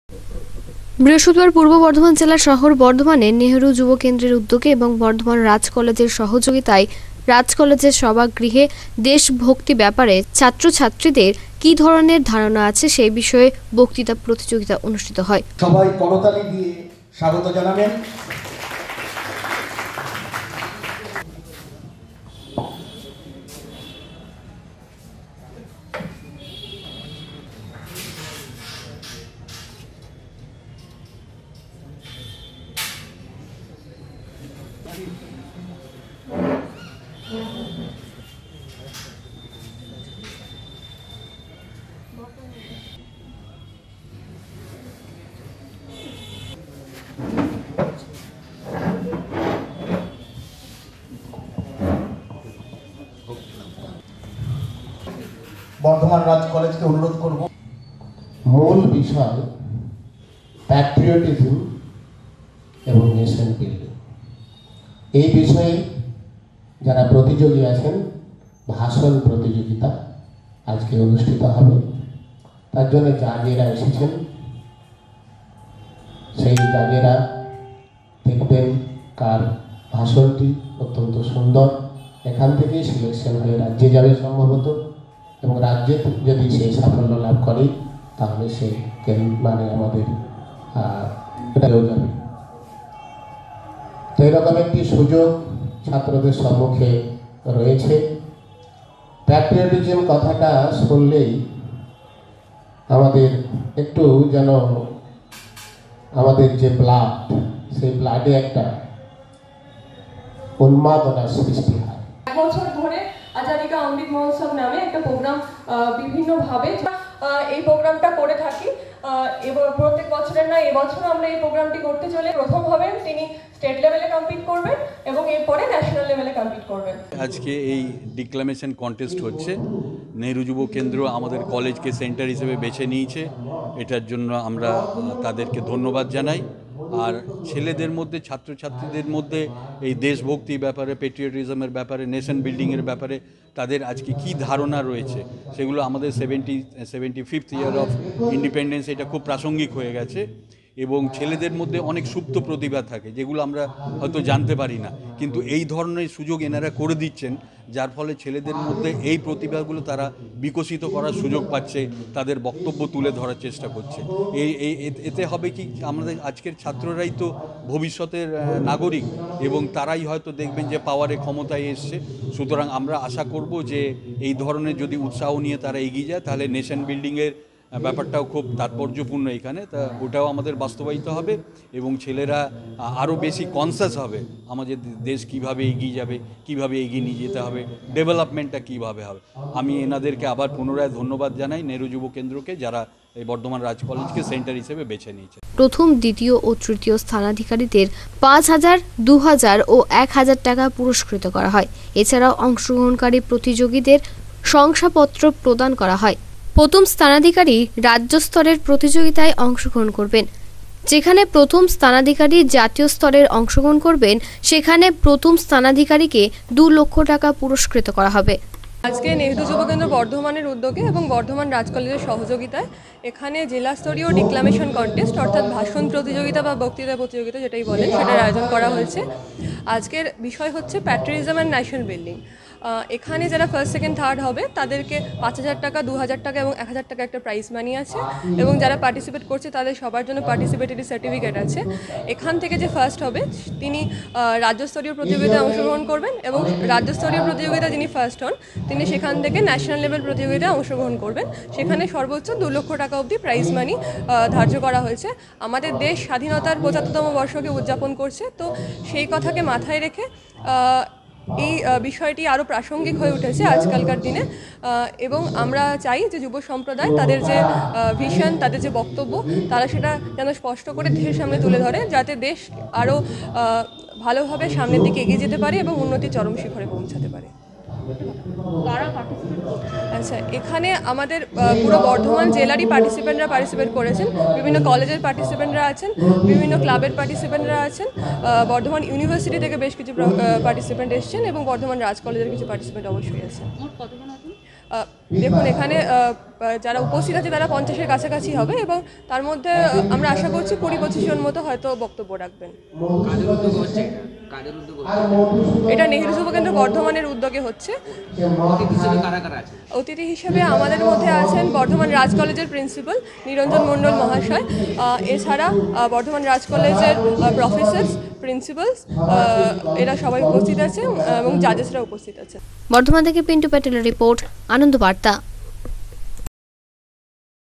বক্তৃতা প্রতিযোগিতা অনুষ্ঠিত হয়
নেহেরু যুব কেন্দ্র ও বর্ধমান রাজ কলেজ এর সহযোগিতায় দেশভক্তি বিষয়ে কি রকম ধারনা আছে সেই বিষয়ে বক্তৃতা প্রতিযোগিতা অনুষ্ঠিত হয়